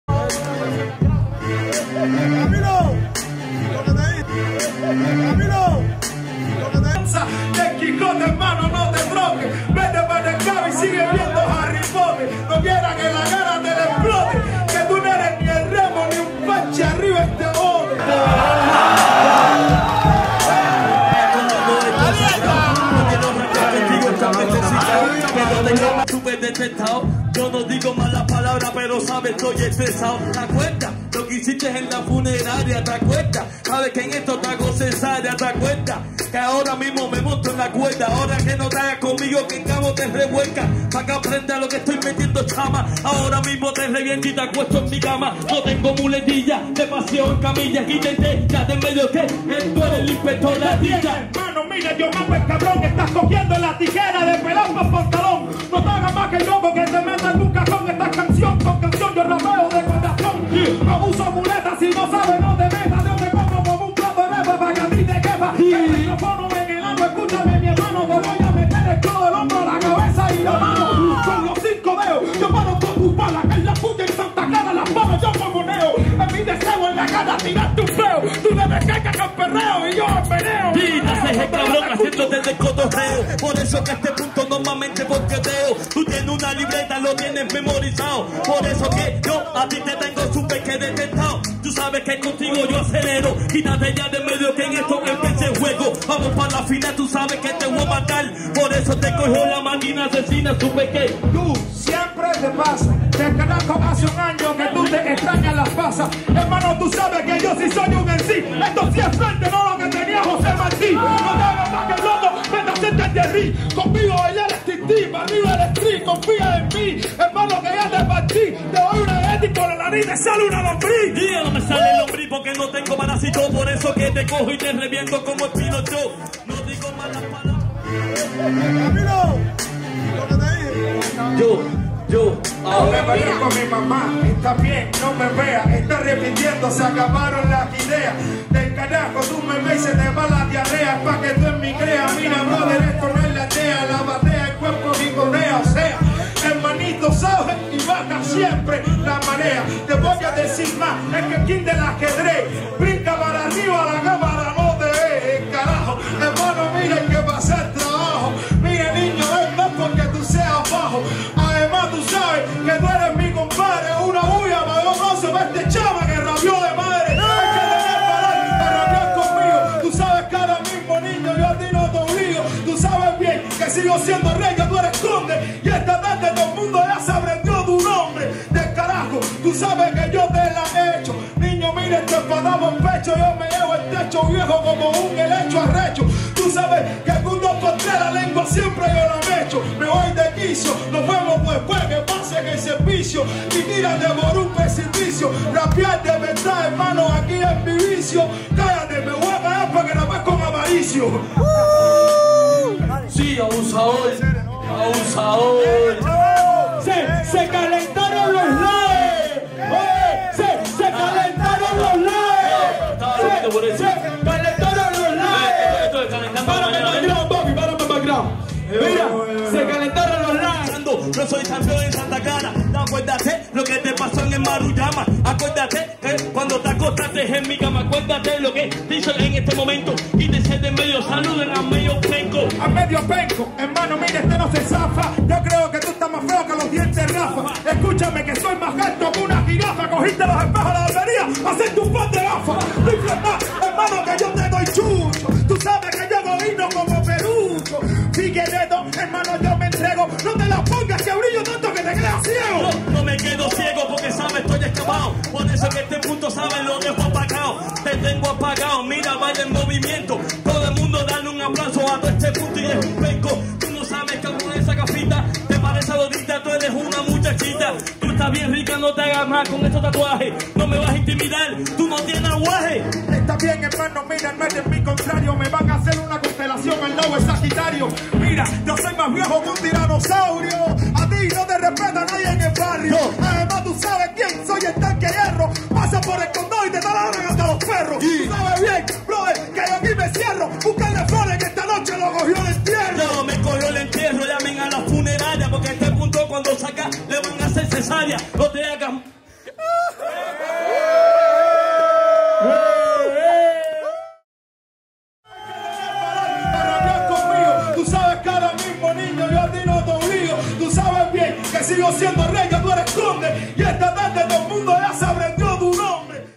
Cuban Freestyle Battle / Hip Hop Batalla de Cuba
While working on a documentary on Cuba, I got a chance to see these guys throw bars in Santa Clara.